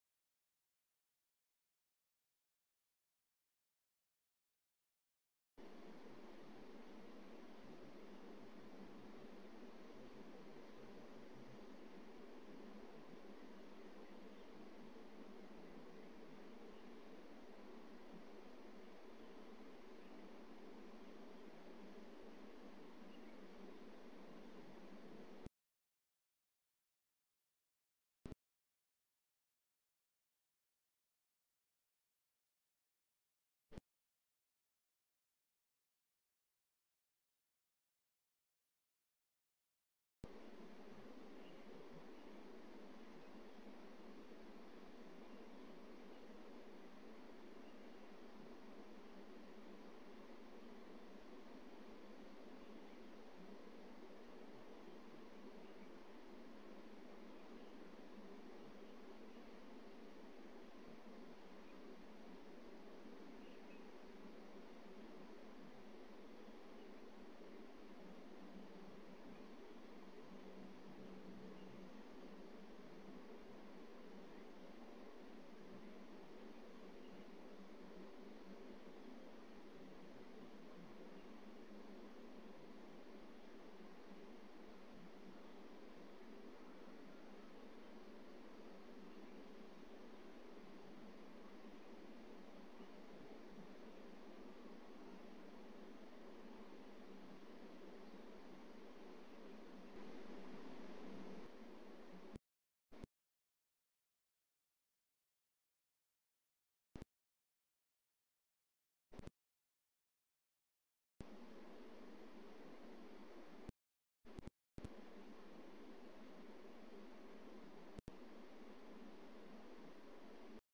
9 10 14 Pan out of Black eagle nesting area post fledge of Jono
Nice pan out looking for Jono.  Beautiful area!